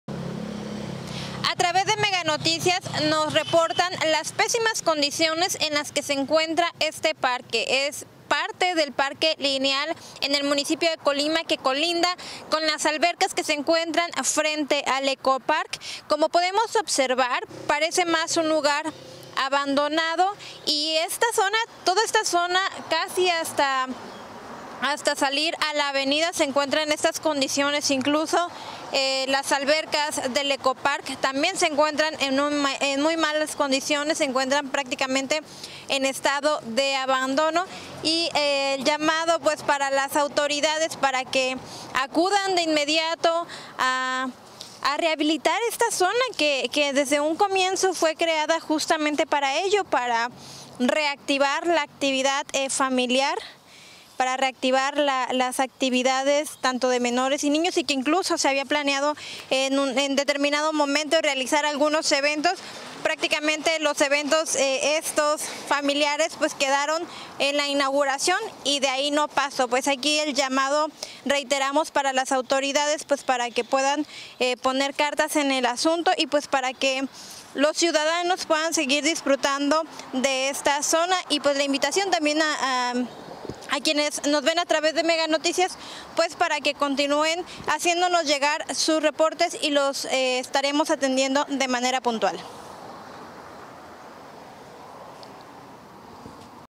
MeganoticiasColima 🌳🍂⚠️En total abandono parque lineal ubicado frente al Ecoparc
En total abandono parque lineal ubicado frente al Ecoparc